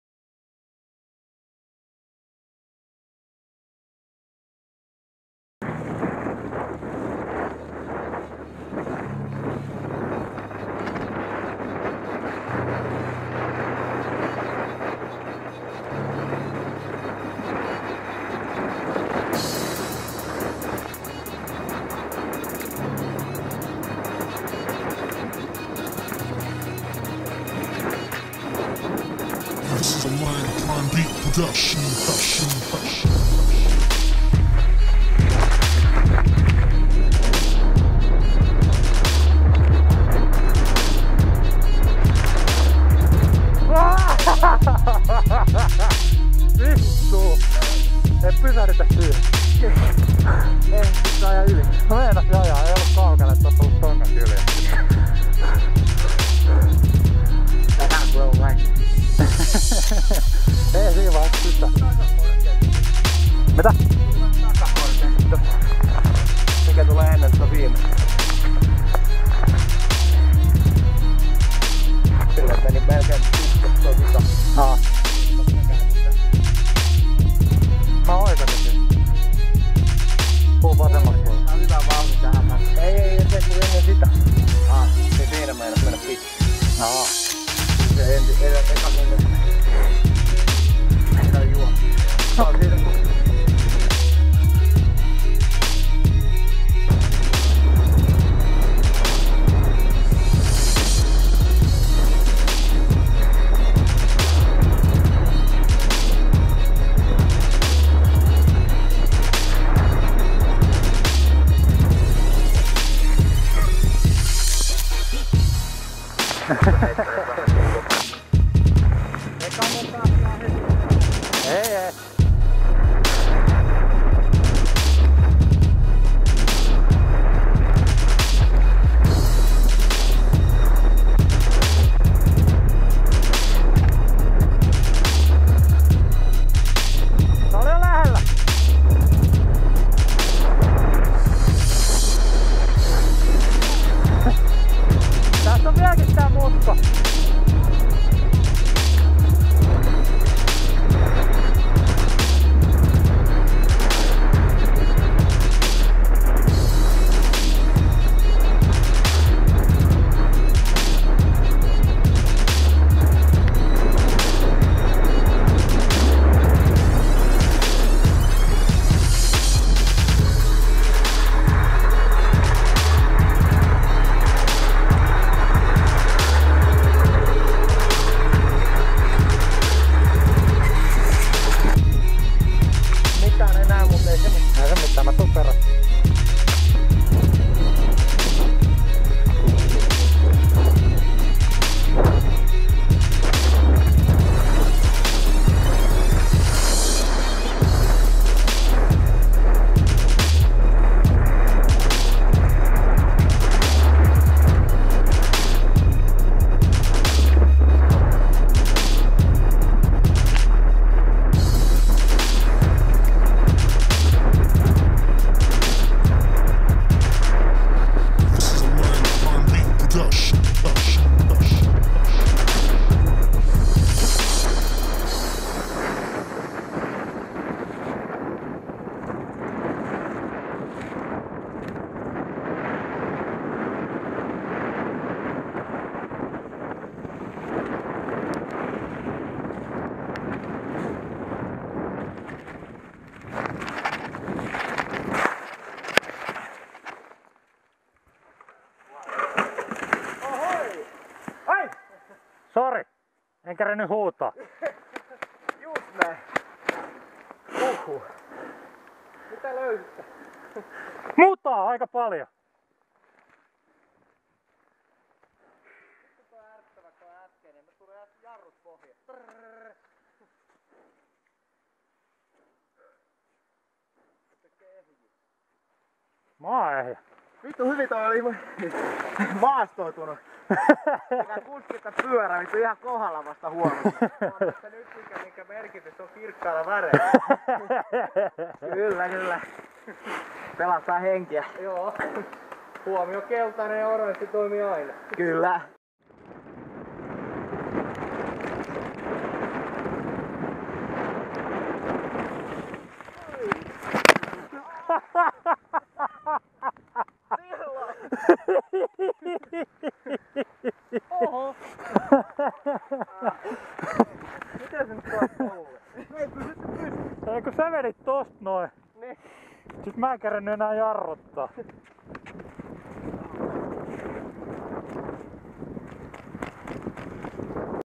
Muddy day @ Calpis Bike Park
Nice and slippery muddy day at local bike park.